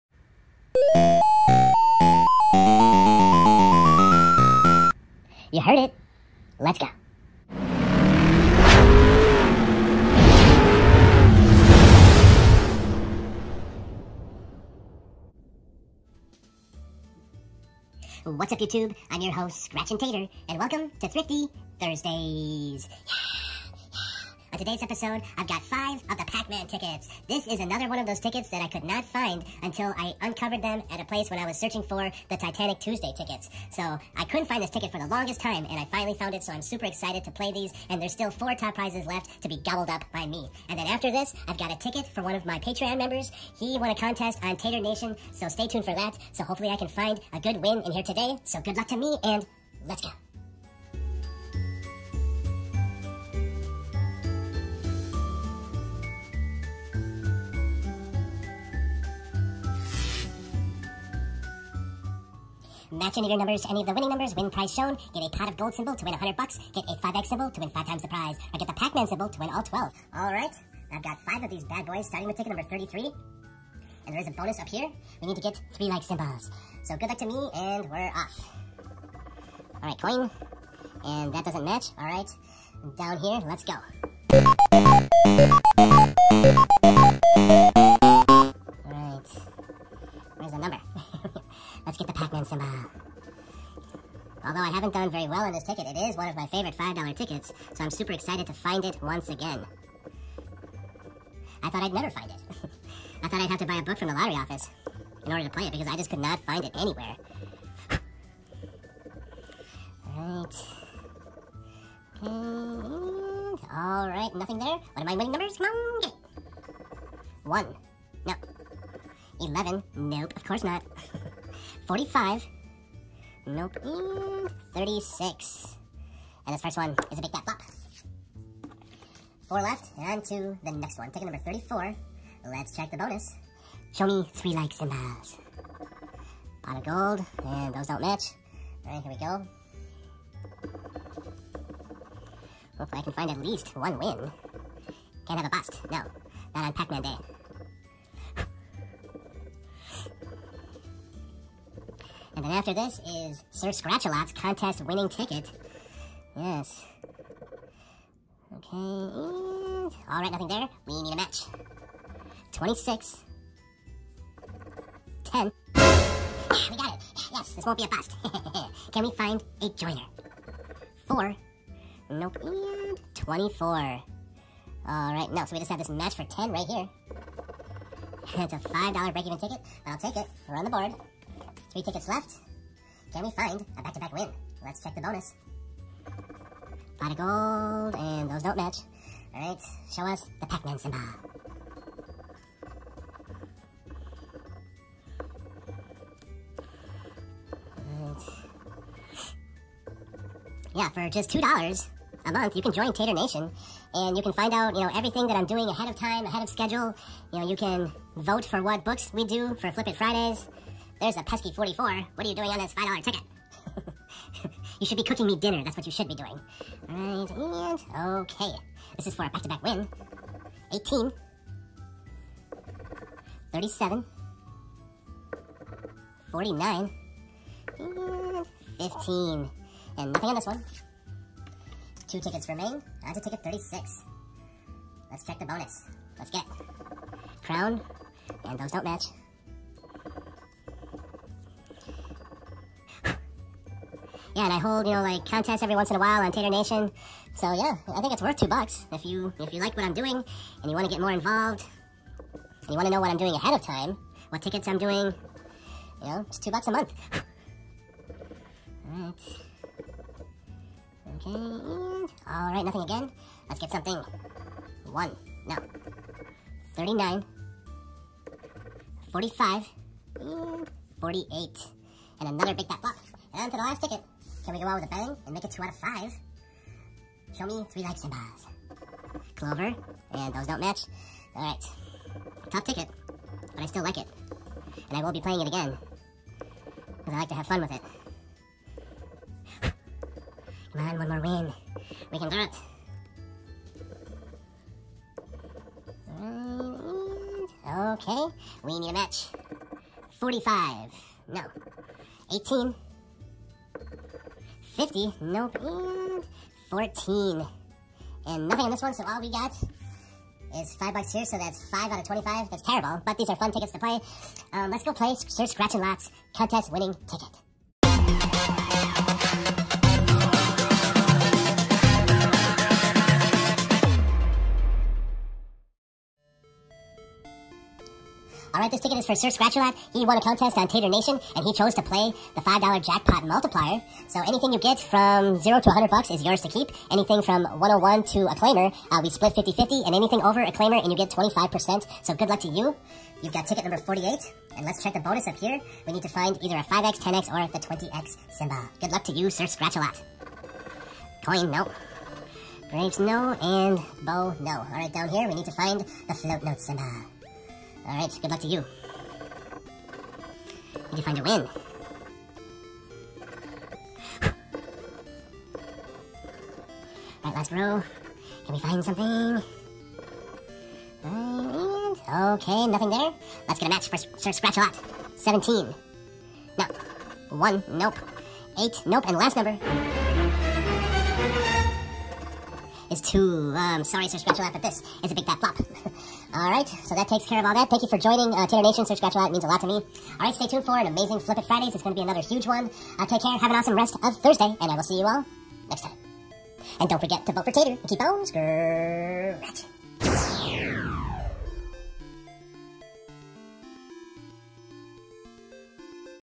PAC-MAN TICKETS!! $25 SESSION! - Florida Lottery Scratchers
Watch Tater scratch (5) of the $5 Pac-man tickets from the Florida Lottery.